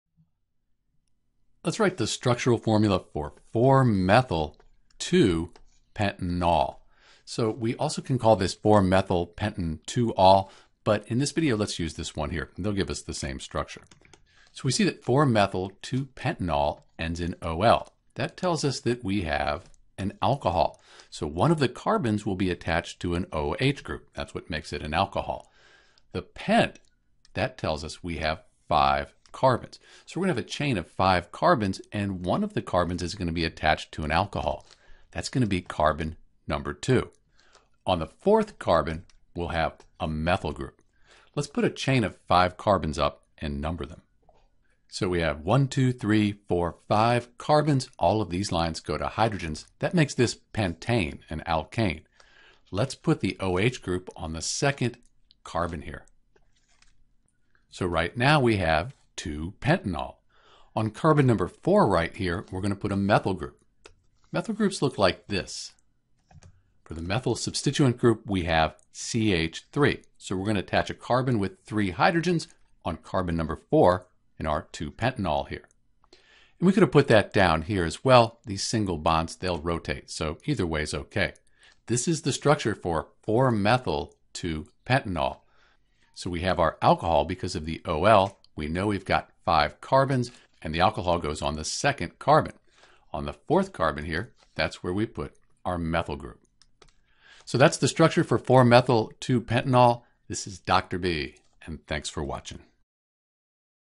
Structural Formula for 4-Methyl-2-pentanol
To write the structure for the organic molecule 4-Methyl-2-pentanol (also called 4-Methylpentan-2-ol ) we’ll start by writing Pentane.  Pentane is an alkane with five carbon atoms connected with single bonds.  Next, we’ll number the Carbon atoms one through five.

Since we have 4-Methyl-2-pentanol (C6H14O) we’ll put an OH on Carbon atom number two.  The OH replaces one of the H atoms on the second C atom in Pentane.  Since we also have 4-Methyl we'll put a methyl (-CH3) group on the forth Carbon.  This gives us the correct structure for 4-Methyl-2-pentanol.

We can also write the structure using straight lines without the element symbols, which is a sort of molecular structure shorthand  This is the skeletal formula for 4-Methyl-2-pentanol.  Finally, we could represent the structure with molecular models to provide further information about the molecular geometry.